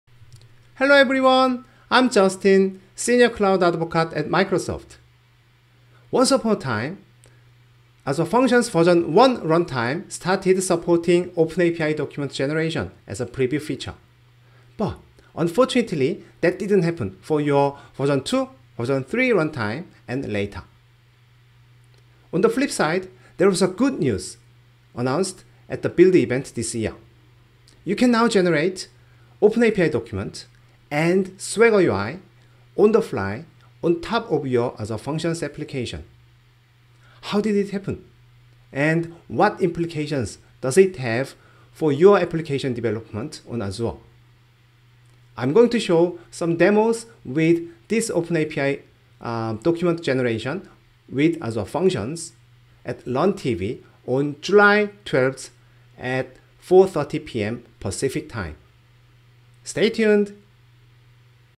Azure Functions: Discover OpenAPI | Learn TV Promo
A long time ago, Azure Functions v1 started supporting OpenAPI document generation as a preview feature. Unfortunately, it hasn't supported for v2, v3 or later.

However, since the announcement at the Build event this year, you can now generate the OpenAPI document and Swagger UI on-the-fly on top of your Azure Functions app.

In this Learn TV session, with my colleagues April and Shayne, I'm going to show some demos about the OpenAPI extension for Azure Functions.